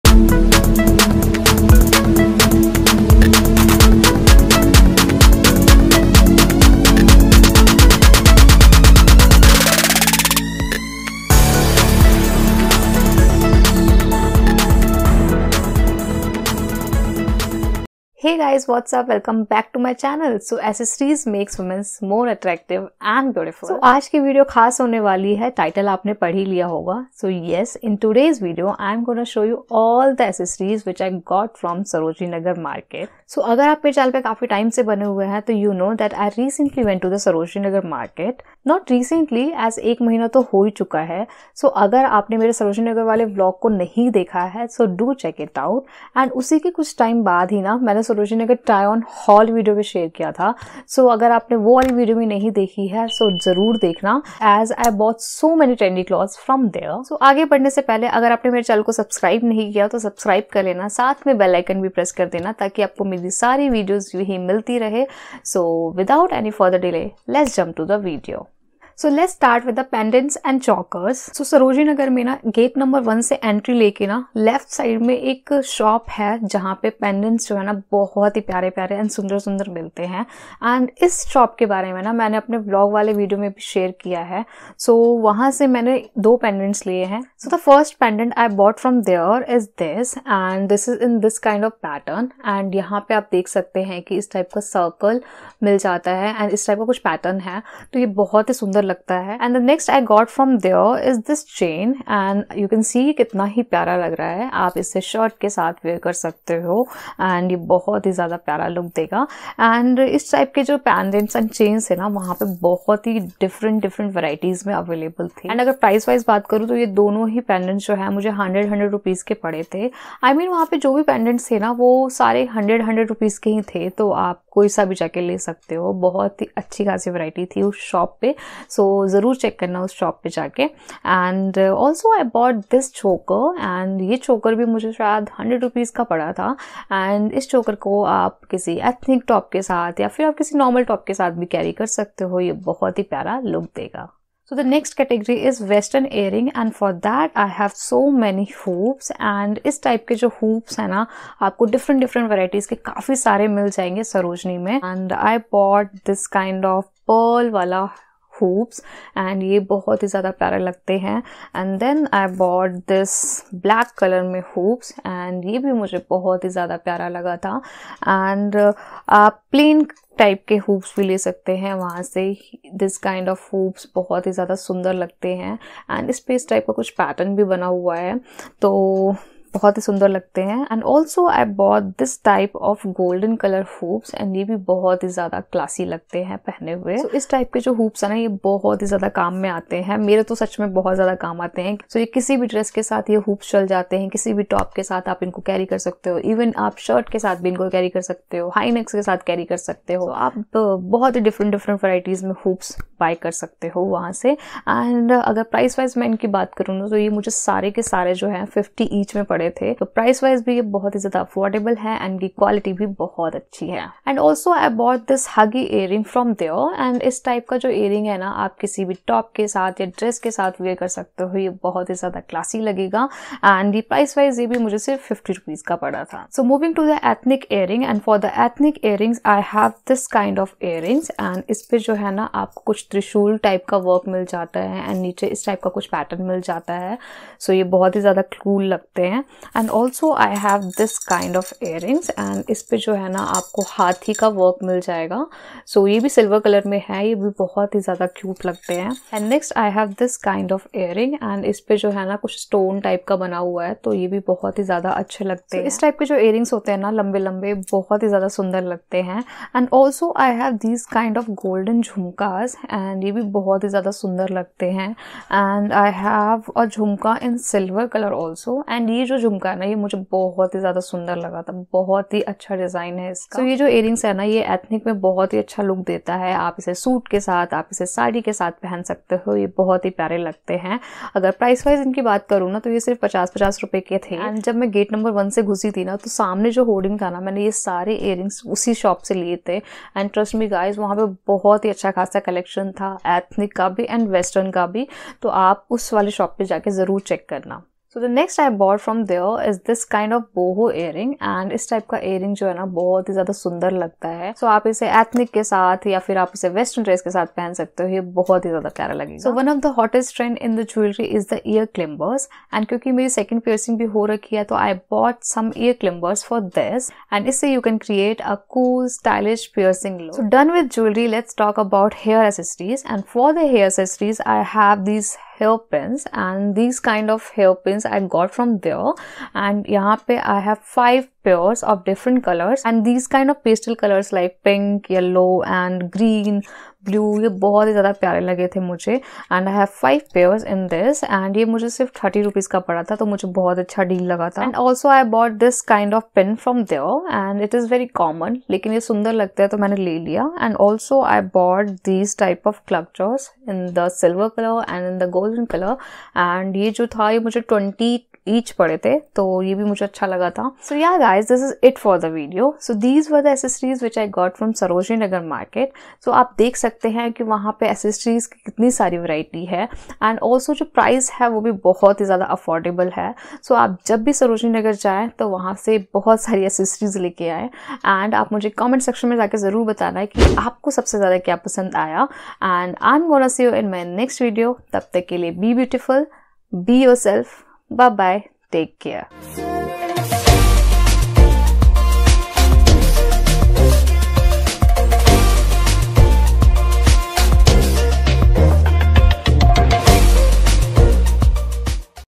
Sarojini Nagar Market Latest Jewellery Haul | Trendy pendants, earrings & hair accessories
and be a part of my family!!

In today's video I will share with you the HAUL of latest jewellery collection of Sarojini Nagar Market, Delhi. I got trendy pendants, earrings and hair accessories at cheapest price from there. And you will definitely love the collection i got from there.

Contents of this video :

00:00 - Intro
01:10 - Pendants and Chokers
02:25 - Western Earrings
03:47 - Ethnic Earrings
04:58 - Boho Earrings
05:10 - Ear Climbers
05:21 - Hair Accessories

I hope you would enjoy watching this video.!!

Copyright
Plz don't use any photos or content without my permission.

Thanks 😊
Lots of love ❤️